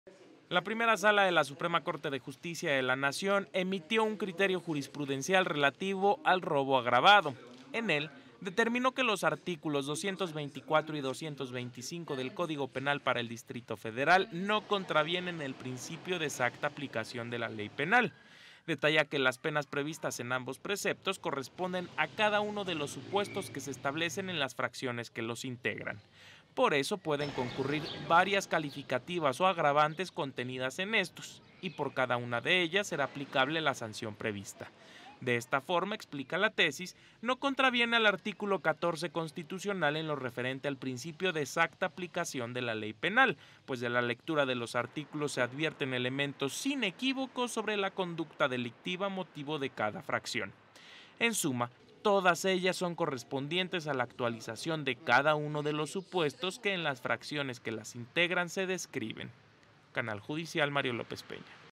La Primera Sala emitió un criterio jurisprudencial relativo al robo agravado
En él determinó que los artículos 224 y 225 del Código Penal para el Distrito Federal no contravienen el principio de exacta aplicación de la Ley Penal.